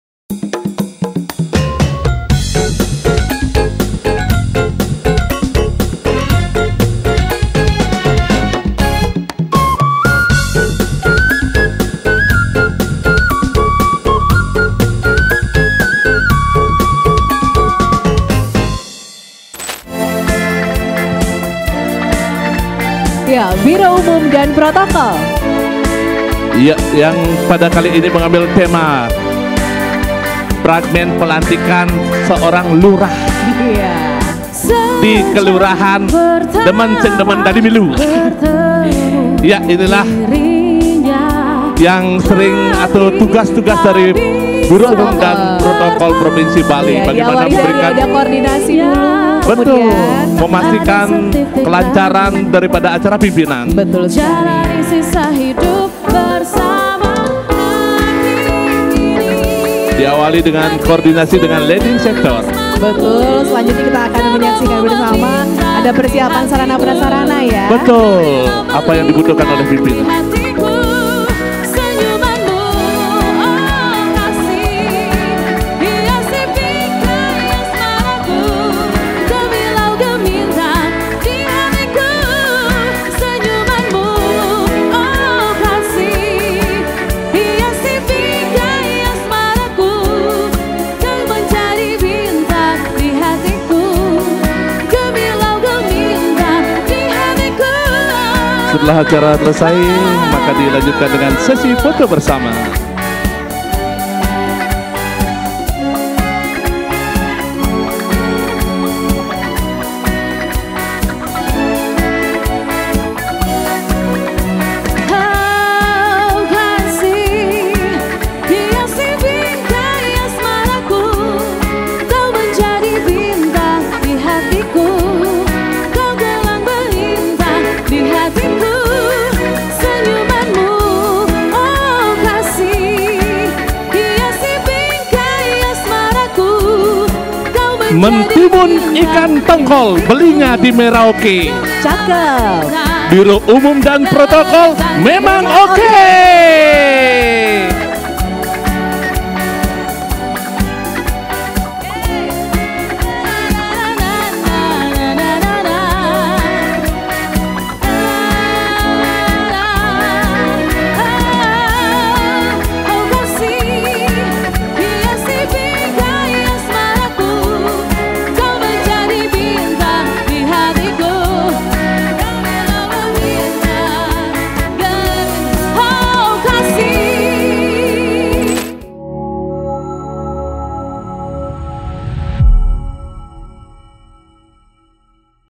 Fashion Show Tim Pelayanan Pimpinan - Biro Umum dan Protokol
Fashionshow oleh tim YanPim (Pelayanan Pimpinan) - Biro Umum dan Protokol Provinsi Bali serangkaian Pembukaan Pameran IKM Bali Bangkit tahap 7 Tahun 2023.

📍Gedung Ksirarnawa, Taman Budaya Provinsi Bali, Art Centre (23 Agustus 2023)